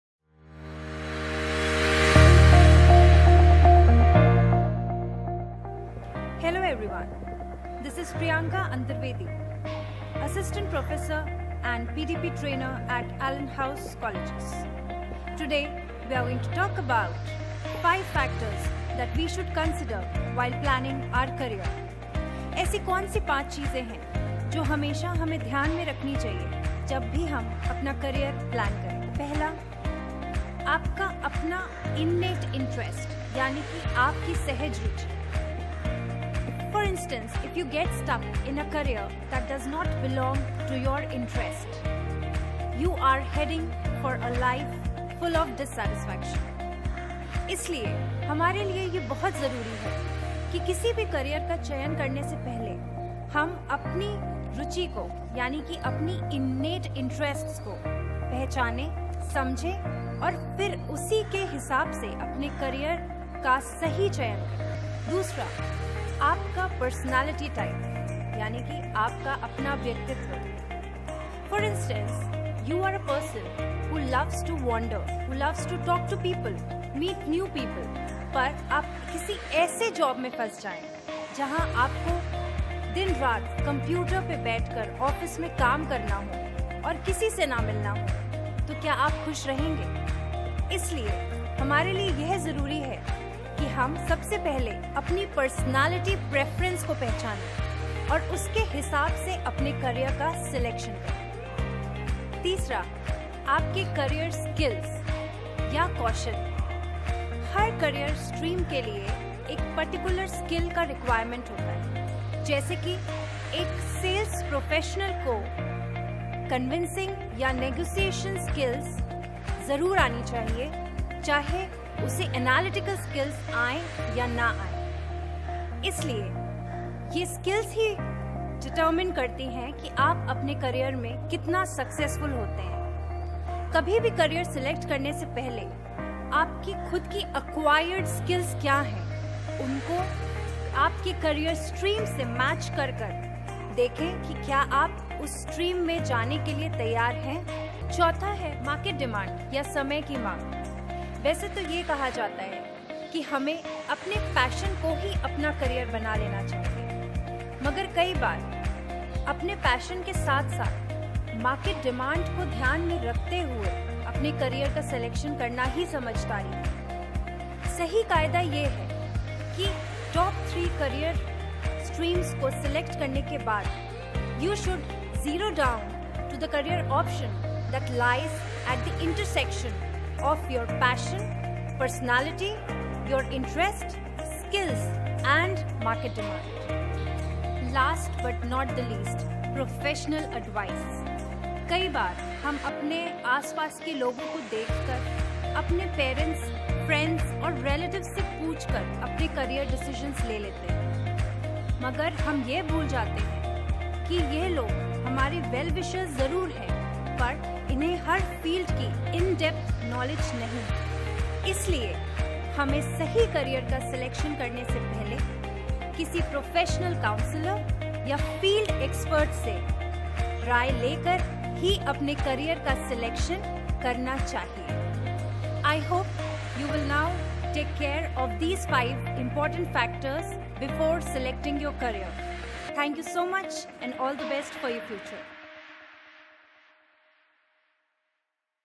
How to Select Career | Allenhouse Colleges | Priyanka Antervedi
With thousands of options available, the task to choose a career that's right for you may seem insurmountable. Fortunately, it isn't.

Watch the video to know 5 factors you should consider while planning your career and increase the chance of making a good decision.